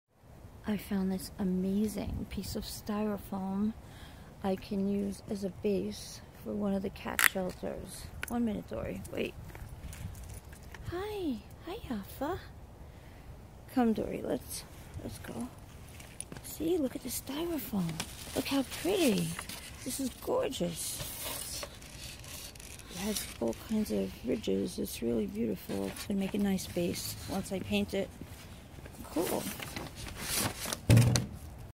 Great piece of Styrofoam found in the garbage
One man's garbage is another man's gold.  I used iresong to build cat shelters it's a great way to recycle styrofoam which is normally not recyclable.